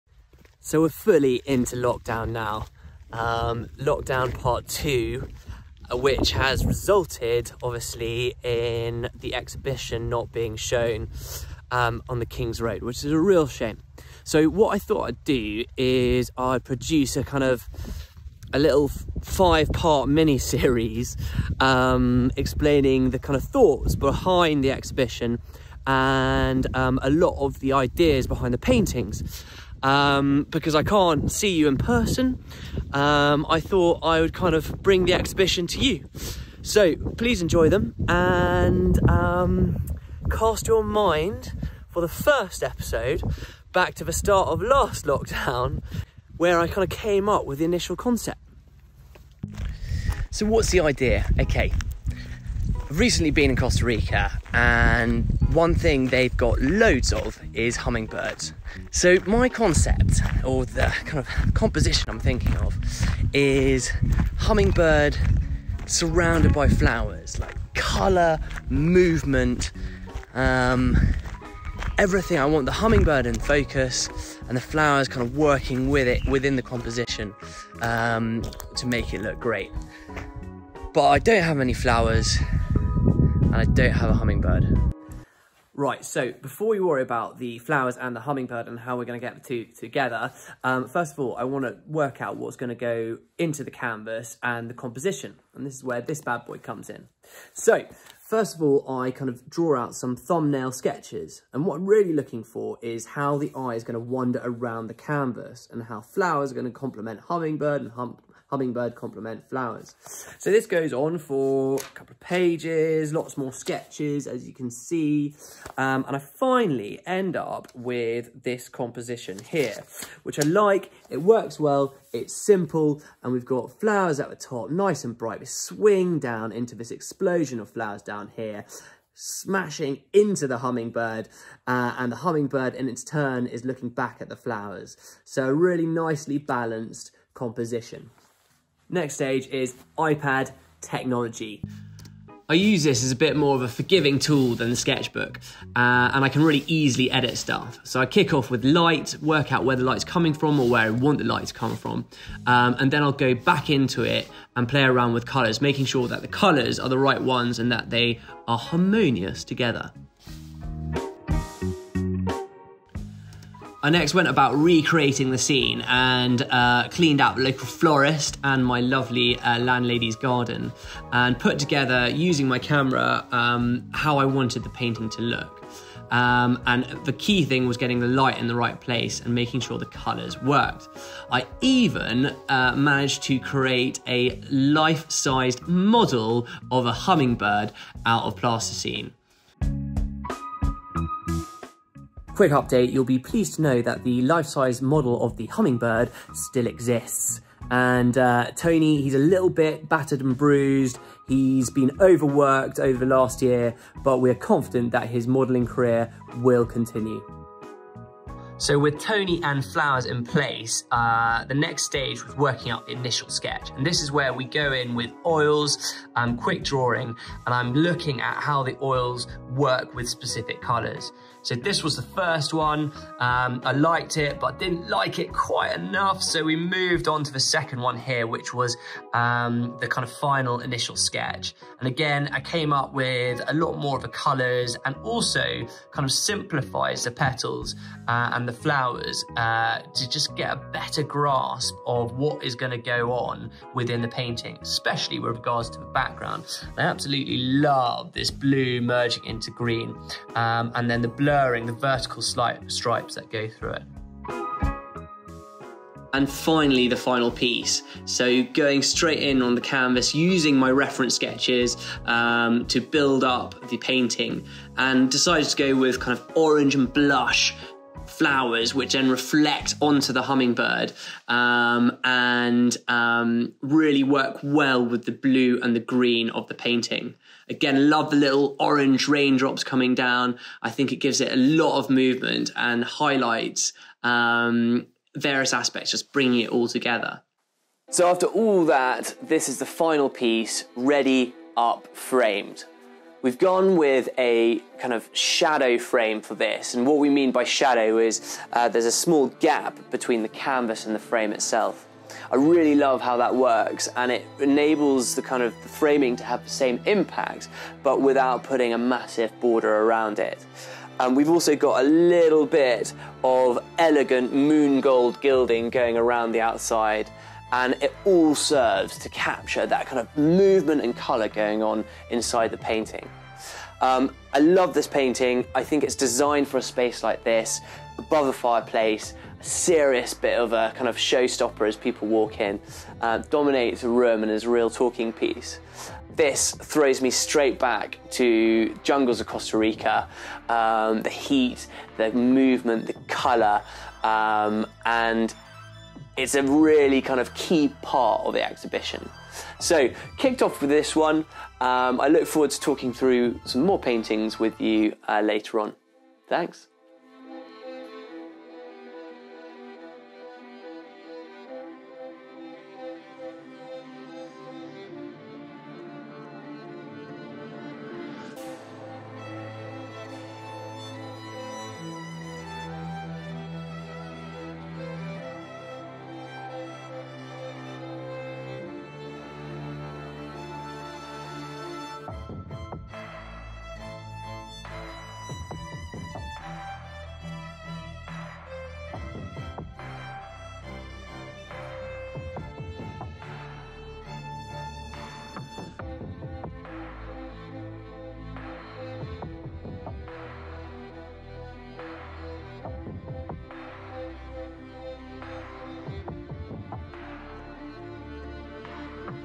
WILD Exhibition Episode One - Hummingbird
A virtual tour of the exhibition WILD.
A look at the process behind the painting 'Jungle Rain'.